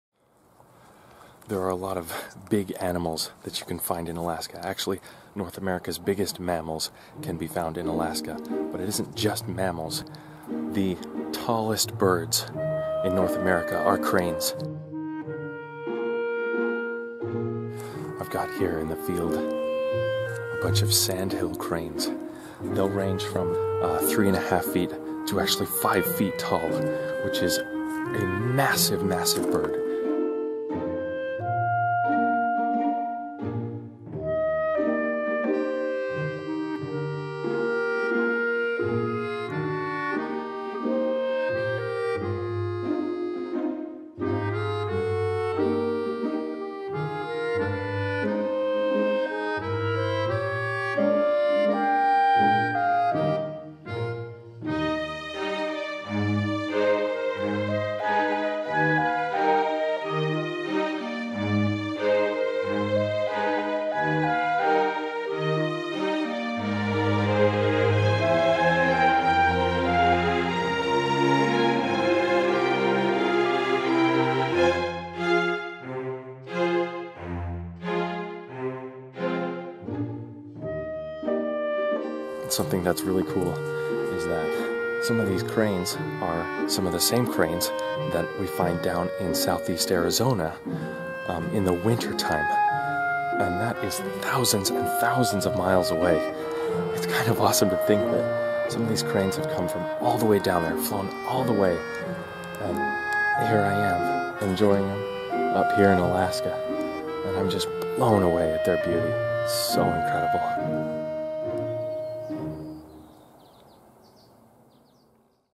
Sandhill Crane! | Spot
SANDHILL CRANE is one of the tallest flying birds in the entire world 🌎, and standing up to 5 feet tall, is second only to the Whooping Crane in North America! They range from Eastern Siberia through much of North America. During winter, they can gather in flocks numbering more than 10,000 cranes! 😱